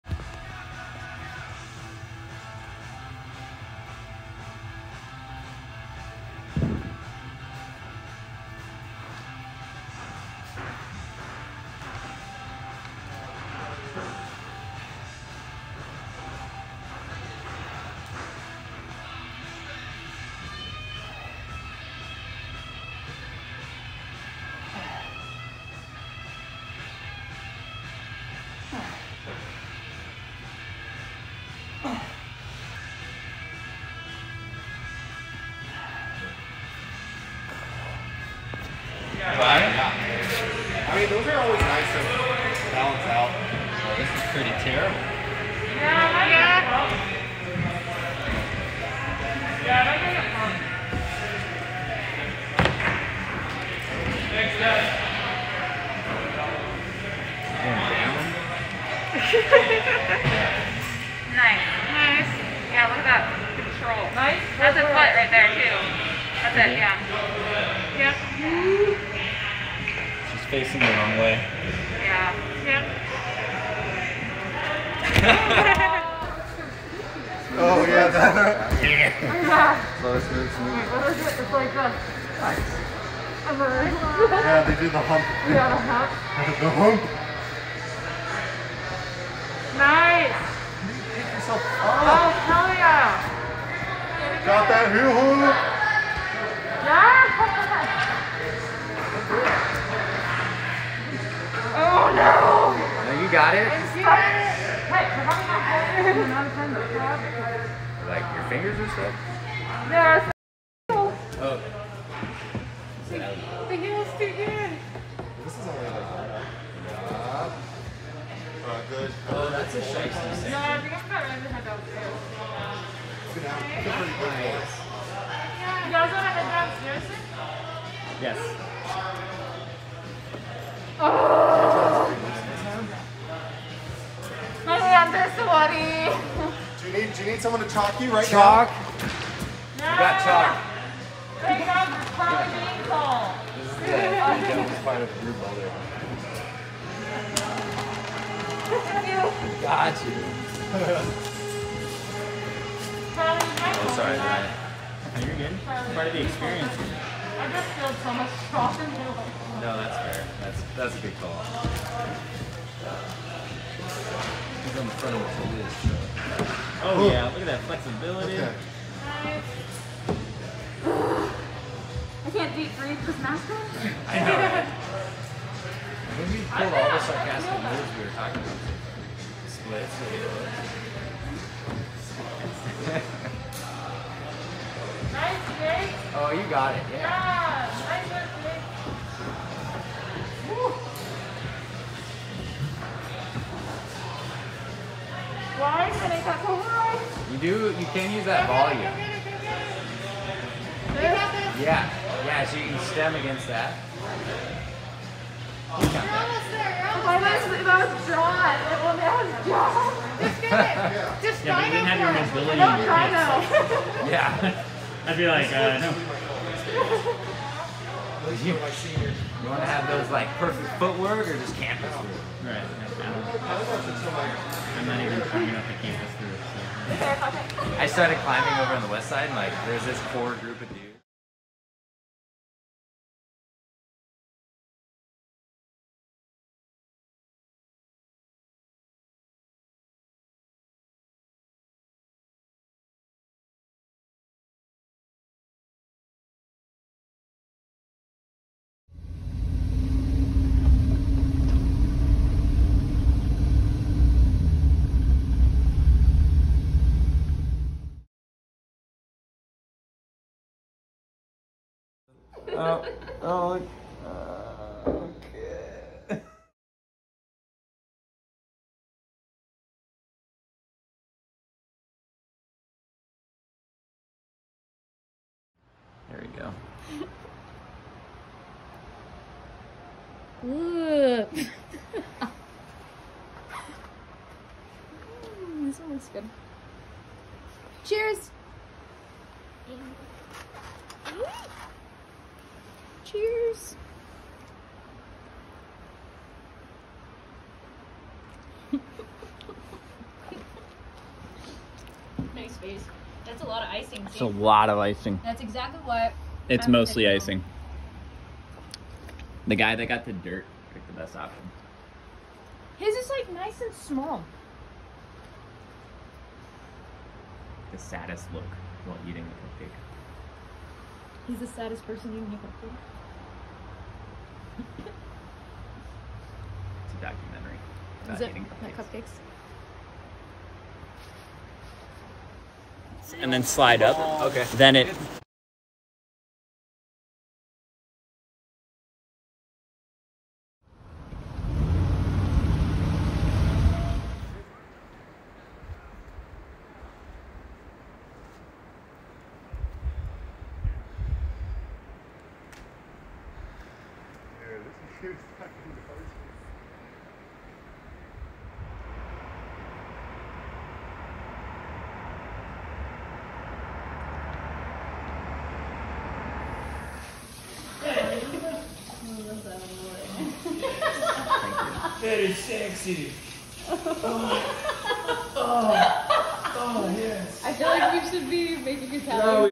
Life is simple and amazing when you surround yourself with the right people
00:00 rings
00:40 Gym Projects
03:53 walking through Levi
04:11 more mountains
04:22 getting old "would you like some tea"
04:29 Water colors with Dev's people
04:33 Ah more mountains
04:35 "Dirt Cupcakes" - happy Birth day Mona
05:43 Damn those colors - that river, those trees